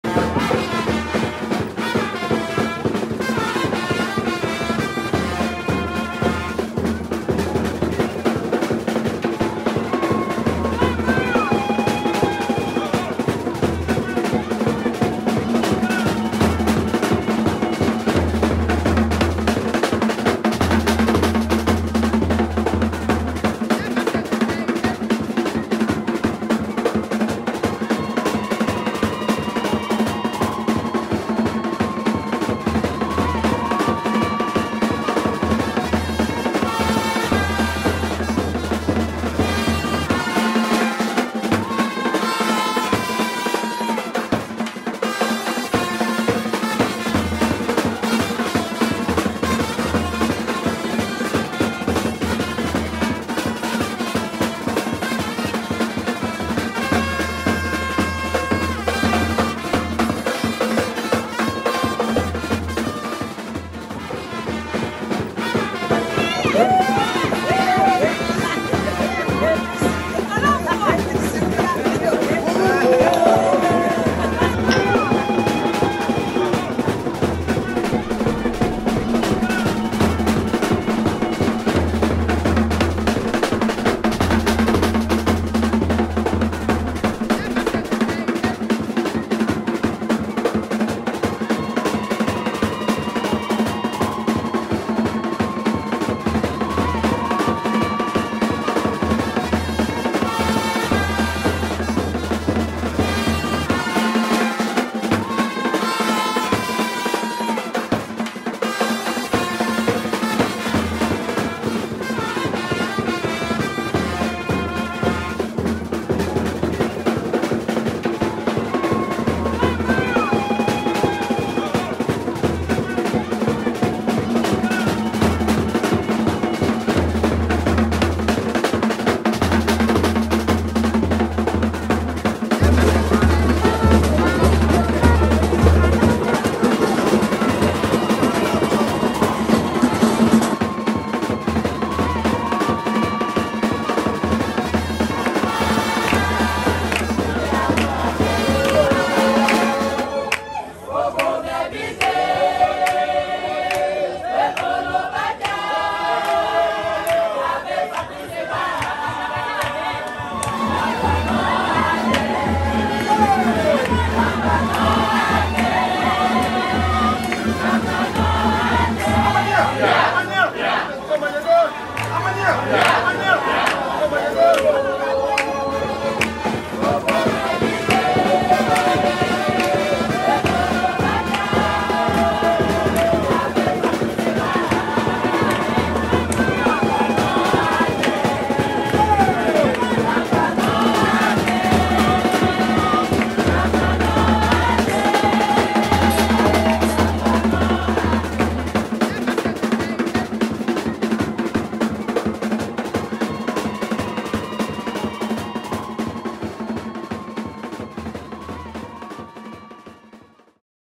Mot du vendredi Edition spéciale - Fête de génération
Aujourd'hui je vous présente une édition spéciale des mots du vendredi.  Un petit reportage tout simple sur la fête des générations du Village d'Anonkoua Konté vécu ce dimanche 25 septembre